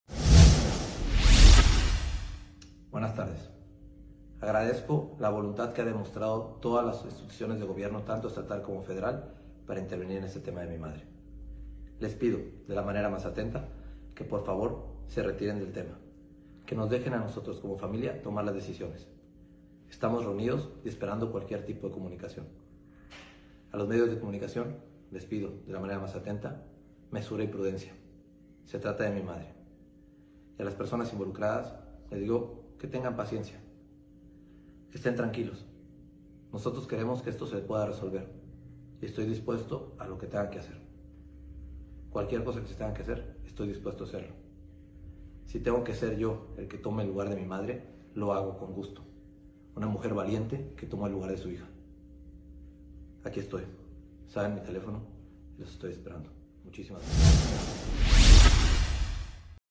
Estoy dispuesto a tomar lugar de mi madre Alcalde de San Andrés
En redes sociales el alcalde de San Andrés Tuxtla, Octavio Pérez Garay, pidió al Gobierno del Estado y a la Federación no intervenir en el secuestro de su madre, Marina Garay, asegurando que su familia se hará cargo del asunto y de negociar con los responsables.